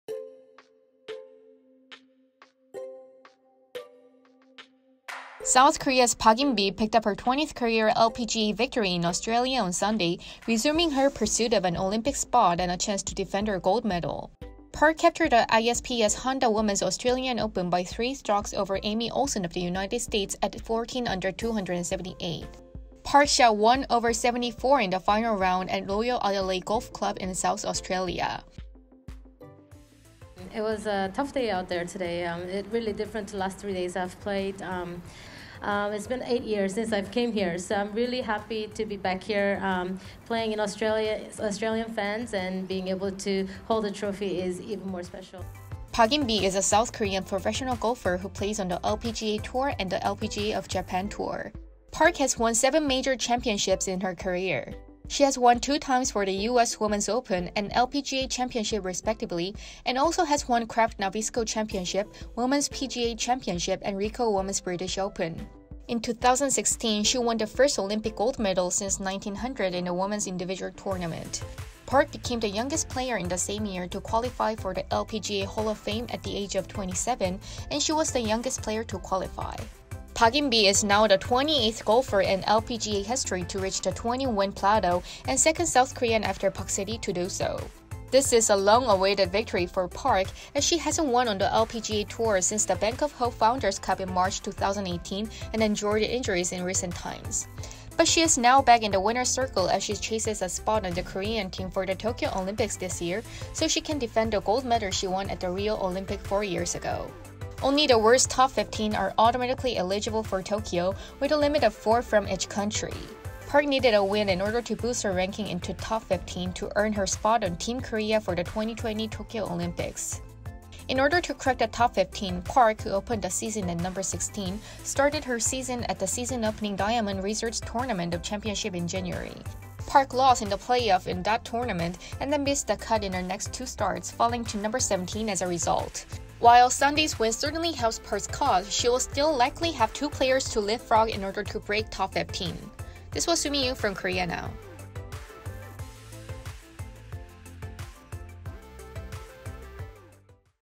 S. Korea’s great Park In-bee wins 20th title at Australian Open
South Korea’s Park In-bee picked up her 20th career LPGA victory in Australia on Sunday, resuming her pursuit of an Olympic spot and a chance to defend her gold medal. Park In-bee is now the 28th golfer in LPGA history to reach the 20-win plateau and second South Korean after Pak Se-ri to do so. Watch this video to find out more.